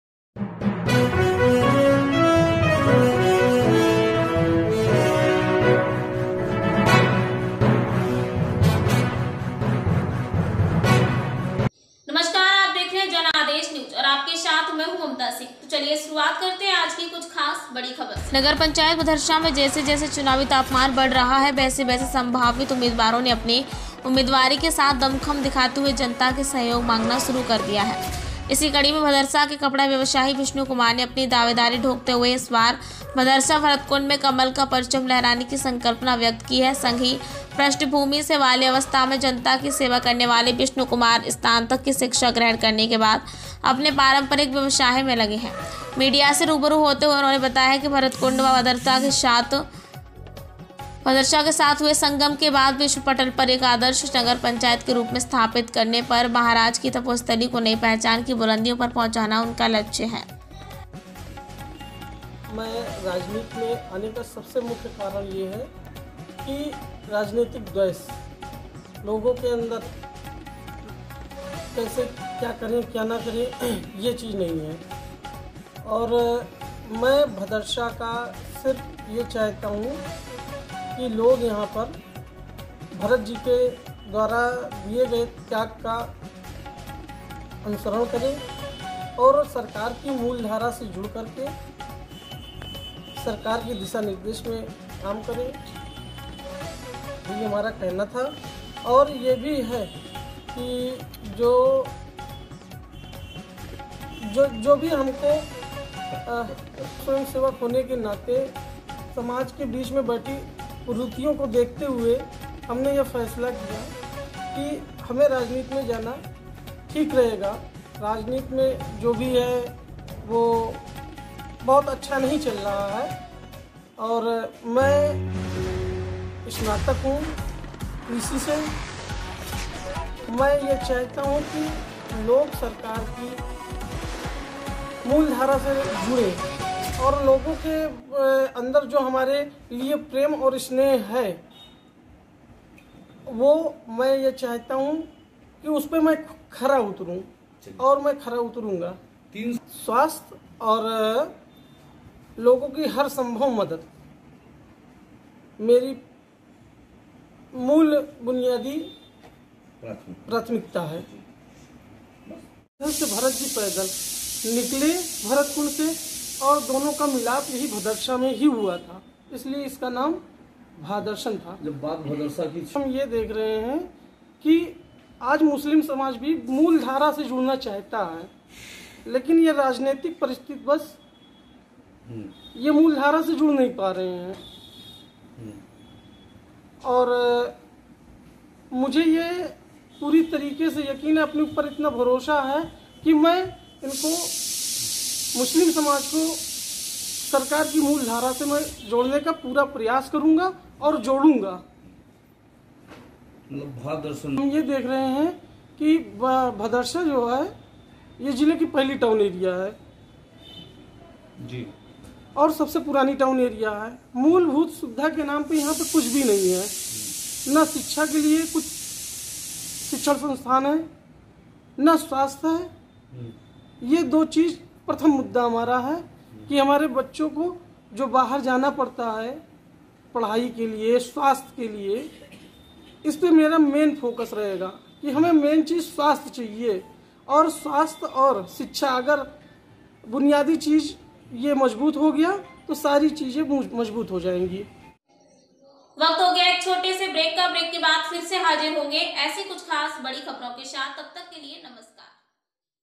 अयोध्या :भरतकुंड भदरसा नगर पंचायत में हिन्दू मुस्लिम एकता की बेमिसाल के साथ उतरे प्रत्याशीupnews
Place Ayodhya.

Polder Slug भरतकुंड भदरसा नगर पंचायत में हिंदू, मुस्लिम, एकता, की बेमिसाल  को करूंगा कायम  =विष्णु कुमार

Anchor नगर पचायत भदरसा में जैसे-जैसे चुनावी तापमान बढ़ रहा है वैसे वैसे संभावित उम्मीदवारों ने अपनी उम्मीदवारी के साथ दमखम दिखाते हुए जनता से सहयोग मांगना शुरू कर दिया है। इसी कड़ी में भदरसा के कपड़ा व्यवसाई विष्णु कुमार ने अपनी दावेदारी ठोकते हुए ।इस बार भदरसा भरतकुंड में कमल का परचम लहराने की संकल्पना व्यक्त की है ।संघ की पृष्ठभूमि से बाल्यावस्था से ही जनता की सेवा करने वाले विष्णु कुमार स्नातक की शिक्षा ग्रहण करने के बाद अपने पारंपरिक व्यवसाय में लगे हैं।
 मीडिया से रूबरू होते हुए उन्होंने बताया कि भरतकुंड का भदरसा के साथ हुए संगम के बाद विश्व पटल पर एक आदर्श नगर पंचायत के रूप में स्थापित करके भरत जी महाराज की तपोस्थली को नई पहचान की बुलंदियों पर पहुंचाना उनका लक्ष्य है ।उन्होंने यह भी संकल्पना दोहराई की भरतकुंड भदरसा  नगर पंचायत में आने वाले समय में हिंदू, मुस्लिम, एकता, की बेमिसाल उदाहरण बनके लोगों को भारत के मजबूत लोकतंत्र की पहचान दिलाएगा। उन्होंने यह भी दावा किया कि यहां पर शैक्षिक संस्थानों की स्थापना करके शिक्षा के मापदंड को बेहतर बनाना भी उनकी प्राथमिकता होगी। मौजूदा नगर पंचायत प्रशासन की कार्यशैली पर असंतोष जाहिर करते हुए उन्होंने दावा किया कि मौका मिलने पर शासन द्वारा गरीबों के निमित्त चलाई जा रही विभिन्न कल्याणकारी योजनाओं को धरती पर उतारकर गरीबों के सशक्तिकरण की दिशा में वह पारदर्शी कदम उठाएंगे।

बाइट विष्णु कुमार भावी प्रत्याशी नगर पंचायत भदरसा